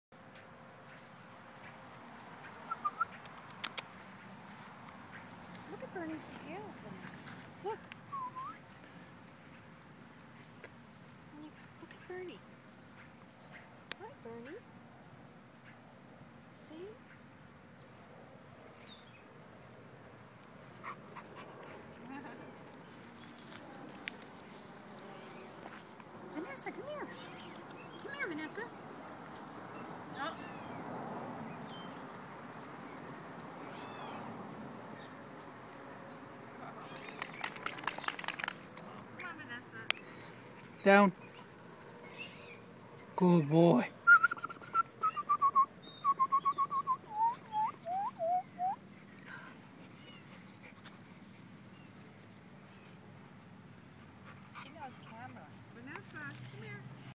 bull dog mauls tiny dog
Bernie Mac Daddy CGC meets a four month old pom for the first time in his yard. Once again this shows it is not the breed but the irresponsible owners.